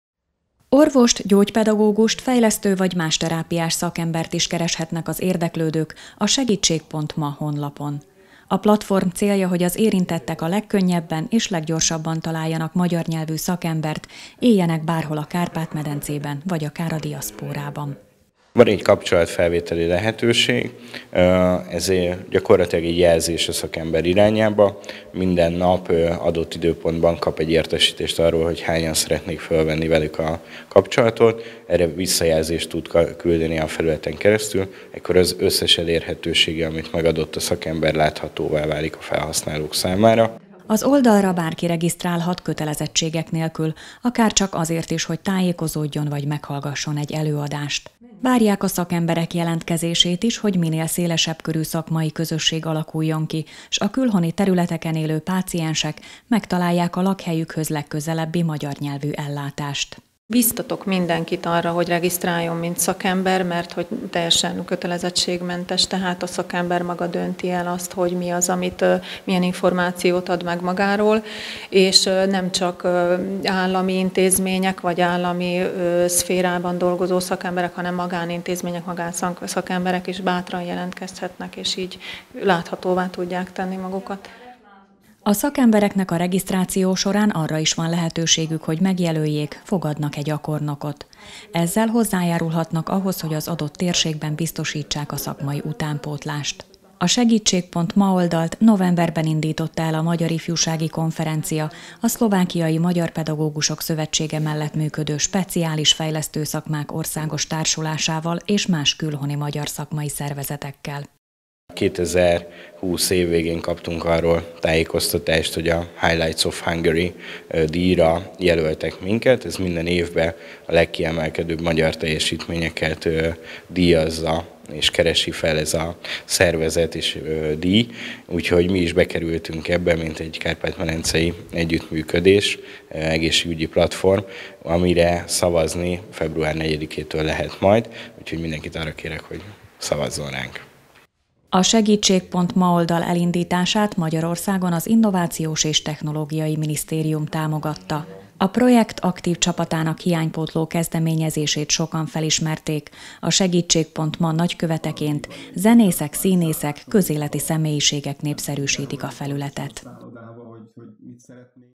Ha magyar orvost keres, próbálja a segítség.ma honlapon
Budapest/Párkány. Orvost, gyógypedagógust, fejlesztő vagy más terápiás szakembert is kereshetnek az érdekődők a segítség.ma honlapon. A platform célja, hogy az érintettek a legkönnyebben és leggyorsabban találjanak magyar nyelvű szakembert éljenek bárhol a Kárpát-medencében vagy akár a diaszpórában.